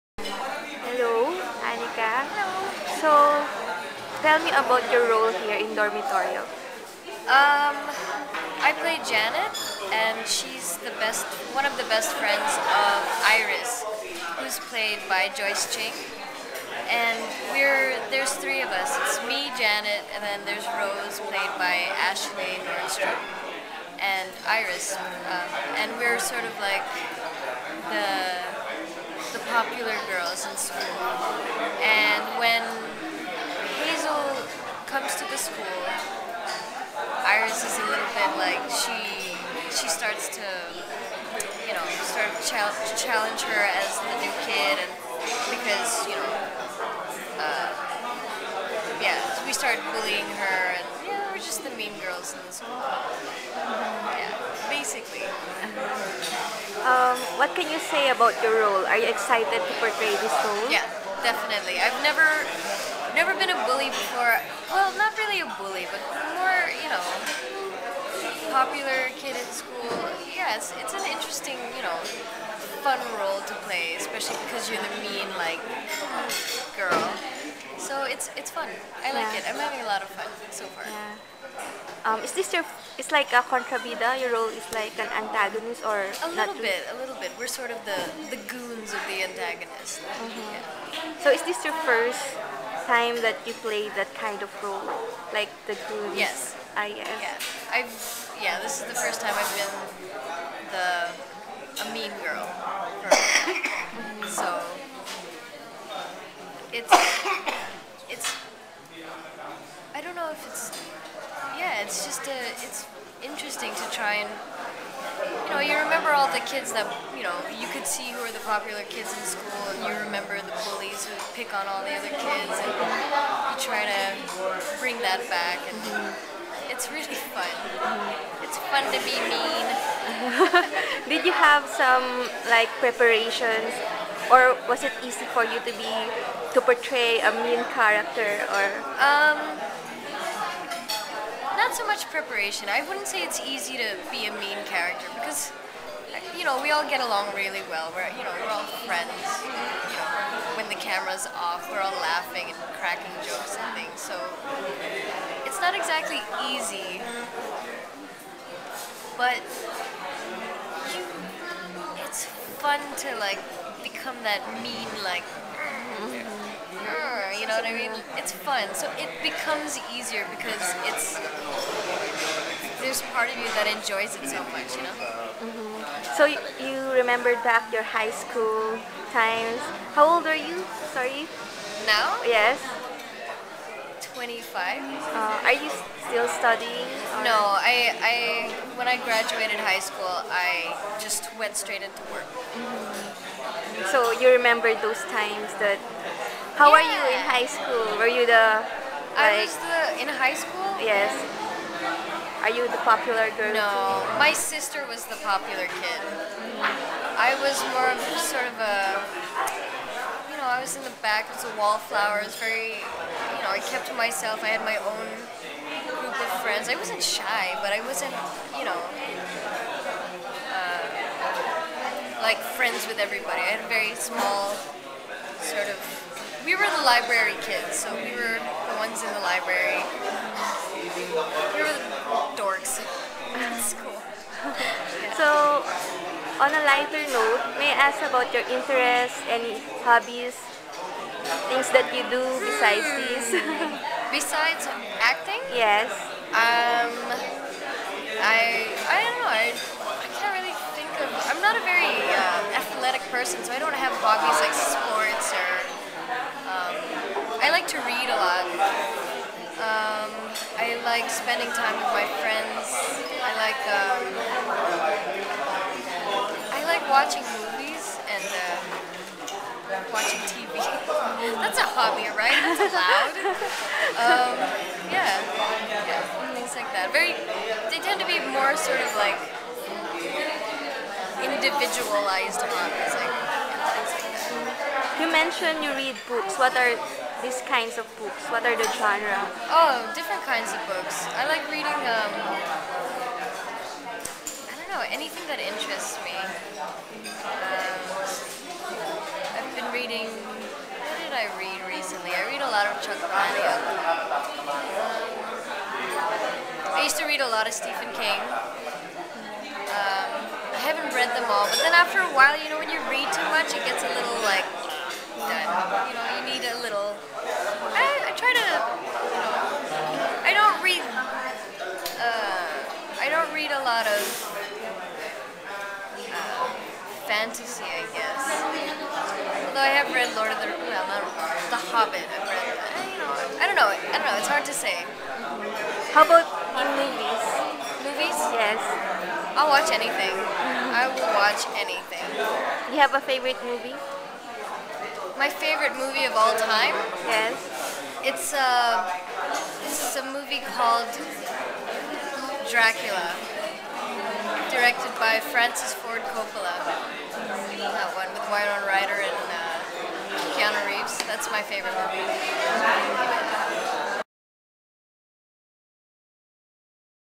Not Seen on TV: Get to know the new Kapuso actress Annicka Dolonius
Kapuso newcomer Annicka Dolonious tells us more about her role as Janet pulido in GMA's newest weekend suspense serye 'Dormitoryo'. Watch this interview and get to know more about her as she explores her bad girl role in this project.